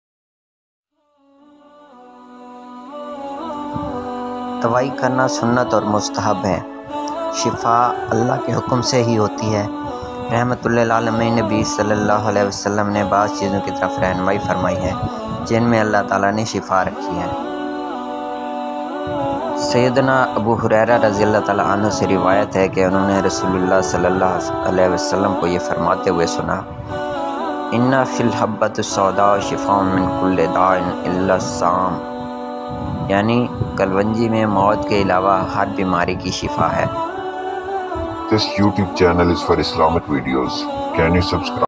Tib e Nabvi | Use of Kalwanji|| Black Seed|| Nigella sativa
learn about health tips in Islam
importance of Kalwanji
habba ul barakah uses